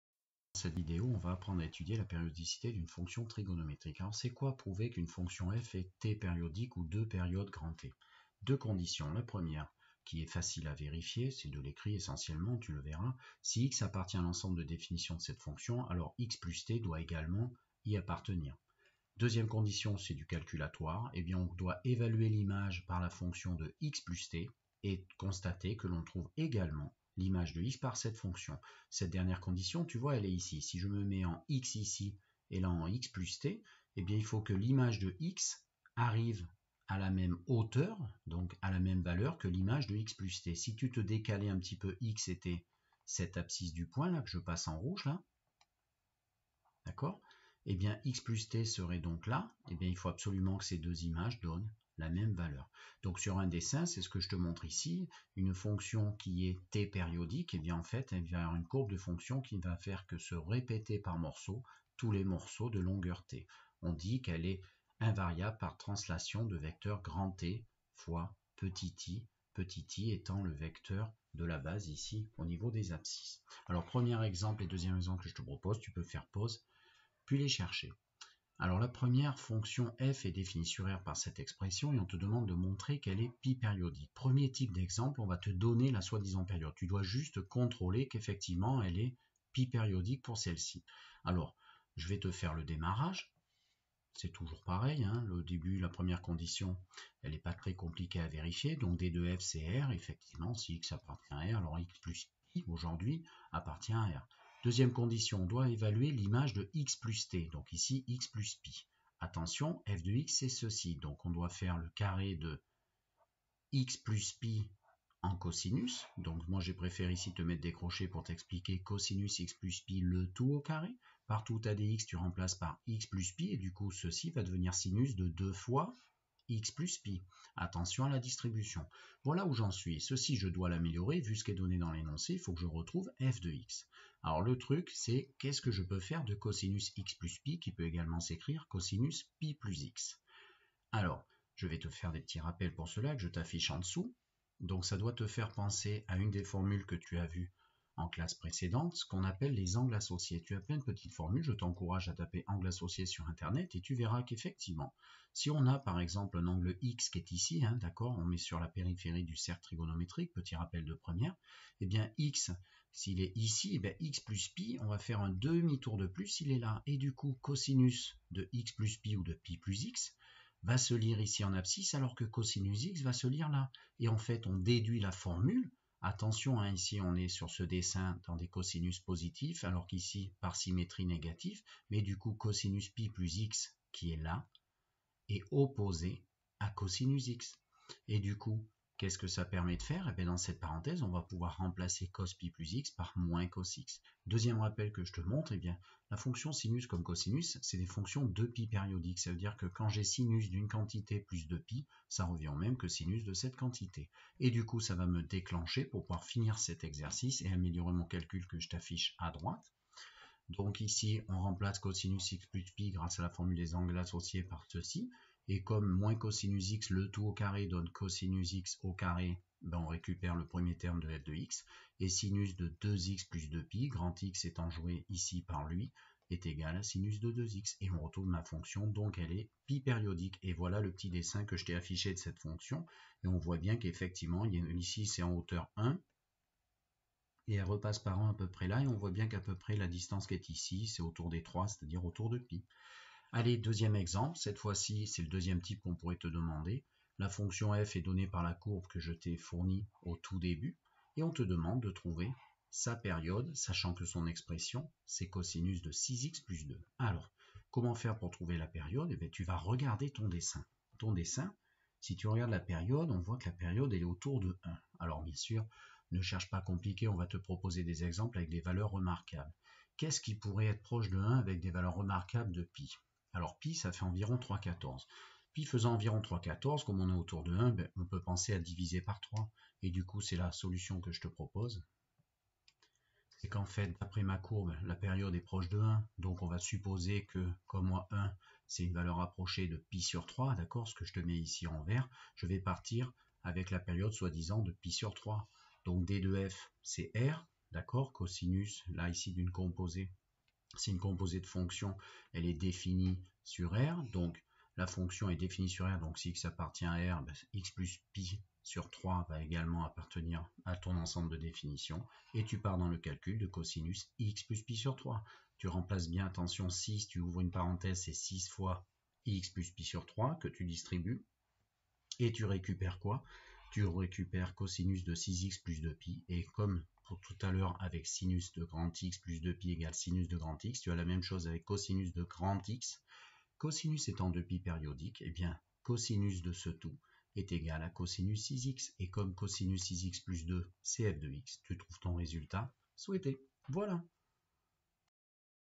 Périodicité d'une fonction trigonométrique. Terminale spécialité mathématiques.
Prouver qu'une fonction est périodique. Les indispensables en moins de 5 minutes en terminale spécialité mathématiques.